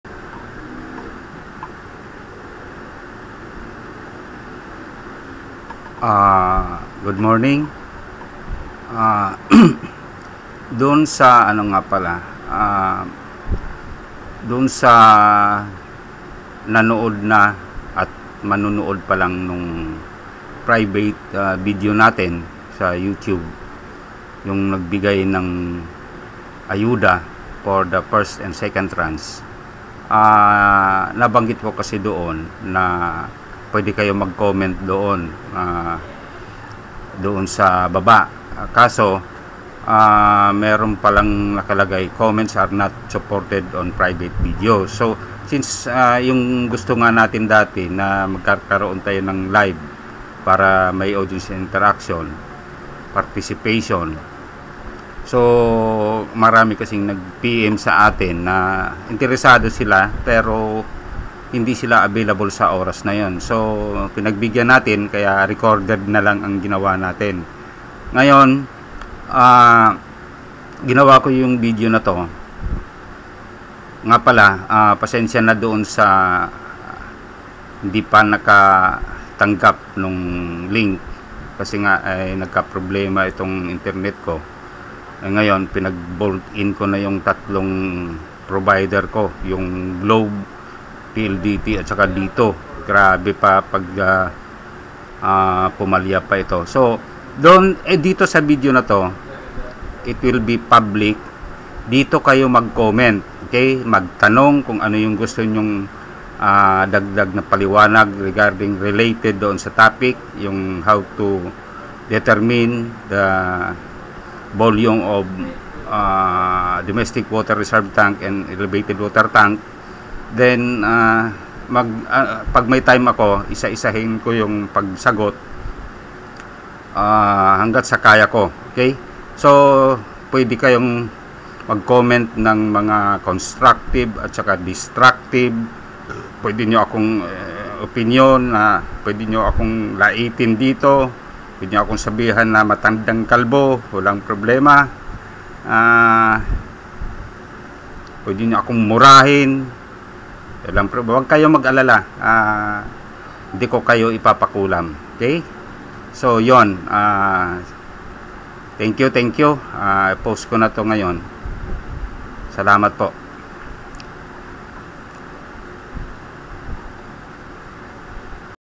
How to determine the volume of Cistern & EWT of a Highrise Building-for comments purposes - public
For those who already viewed & interested to view our Private Video regarding How to Determine the Volume of the Domestic Water Reserve Tank & The Elevated Water Tank of a Highrise Building, dito kayo mag comment, ask questions, etc.